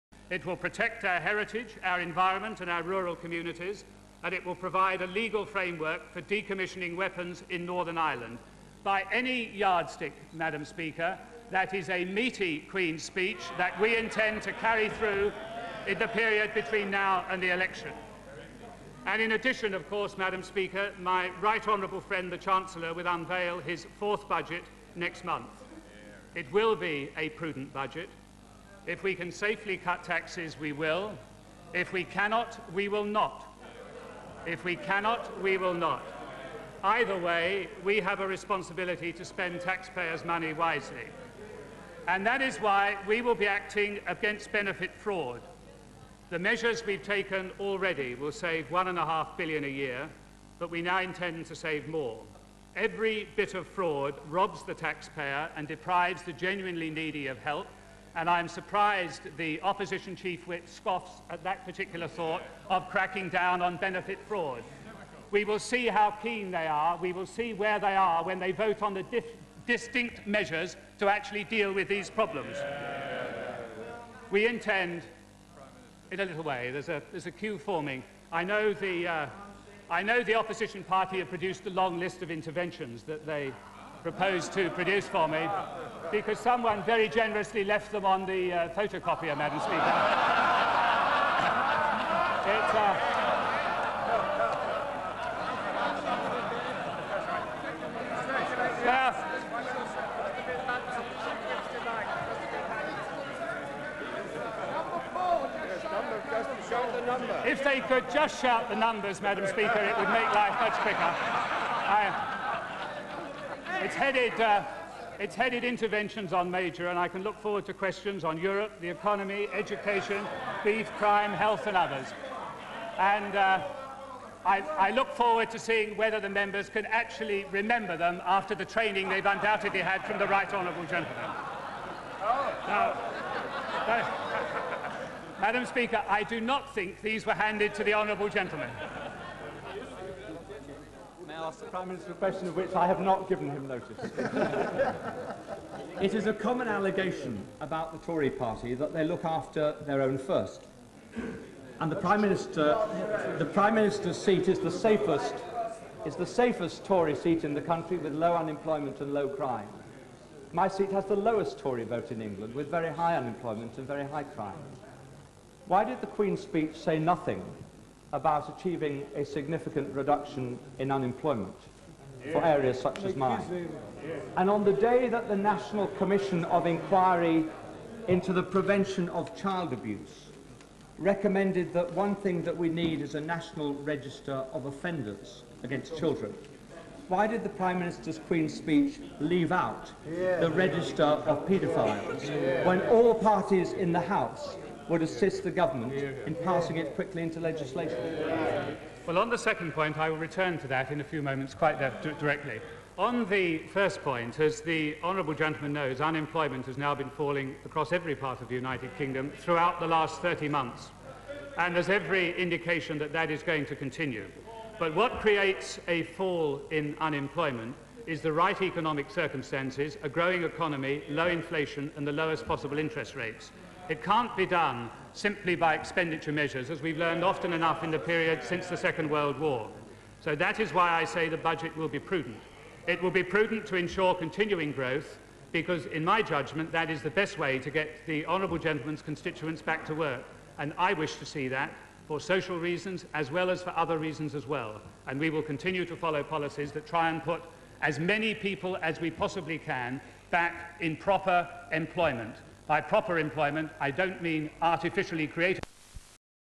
John Major, PMQ, Westminster, 1996
John Major on the state of the Nation